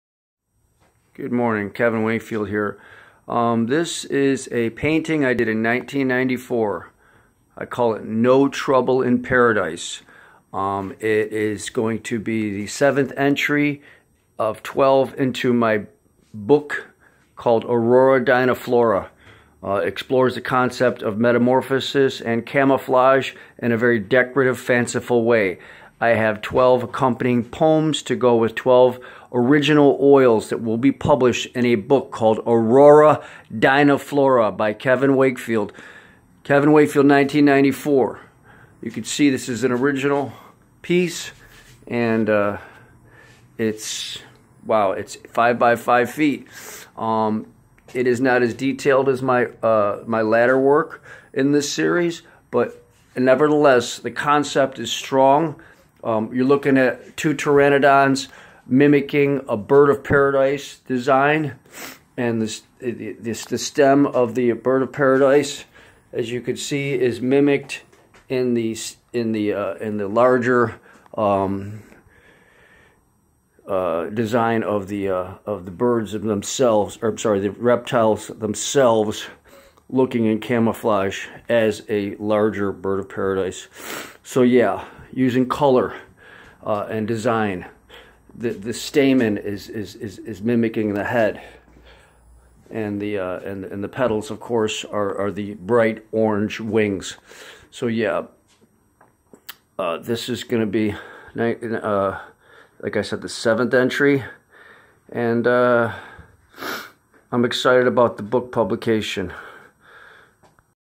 "No Trouble in Paradise"
Pteranadons in cammflague mimmicing the floral environment